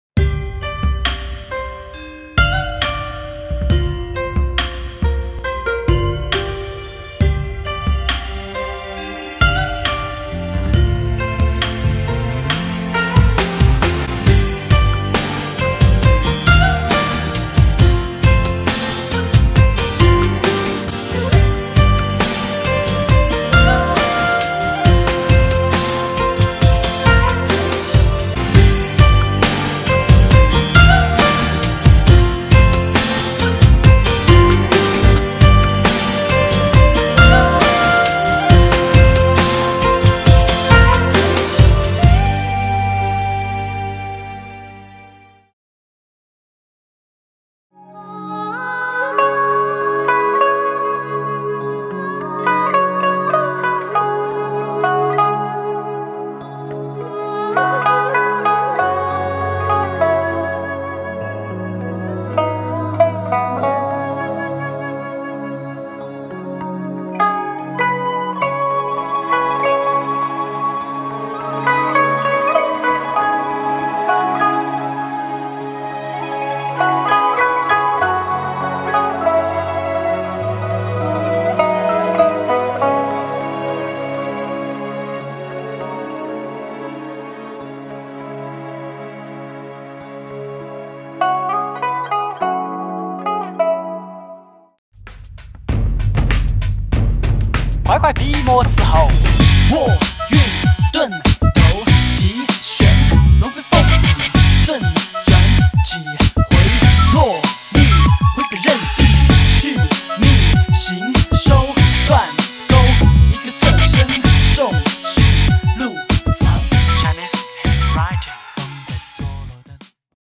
Chinese Calligraphy - The Writing of "Tao"
A project work of Digital Filmmaking course - Making An Instructional Video.